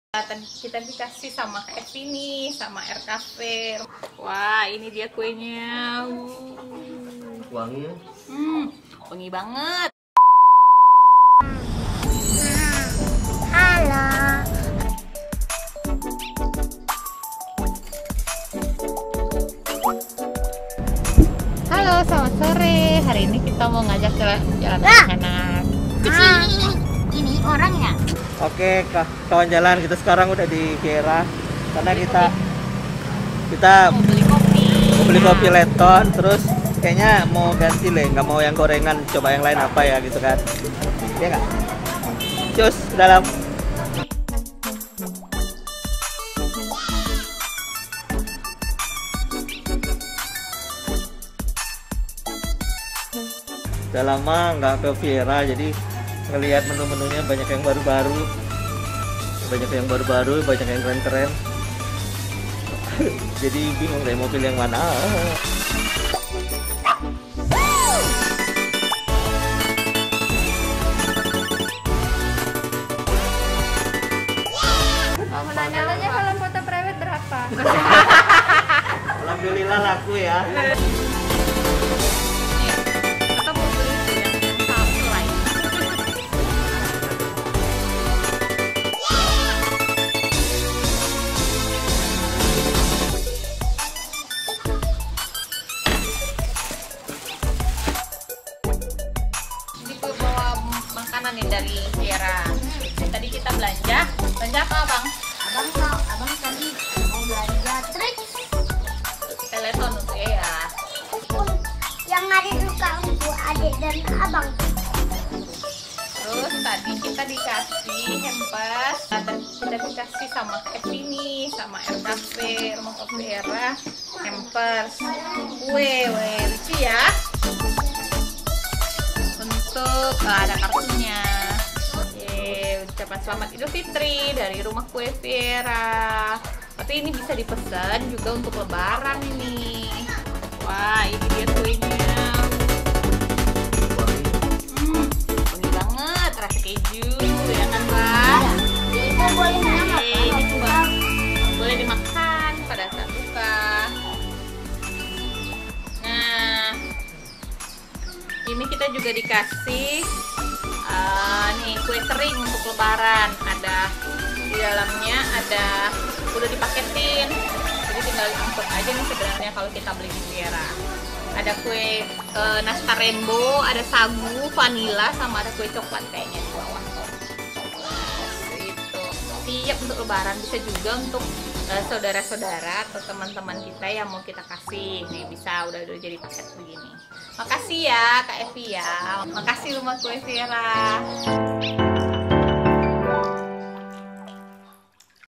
dailyvlog Dapat hampers dari Rumah Kue Viera Pekanbaru
beberapa waktu lalu ngajakin bubu @fitriahasanah_ica main ke @rumahkueviera , pas nyampe sana, ternyata udah pada ready hampersnya!!!
selain liat hampers, saya dan bubu belanja2 cemilan dan kopi leton buat nemenin kalau kerja
sambil ngedit video, enaknya ditemenin cemilan dari Rumah kue Viera. deket soalnya dari rumah
enjoy daily vlog saya dan bubu lainnya di playlist IGTV kami: "bubuyayah" yaaa ya genk!!!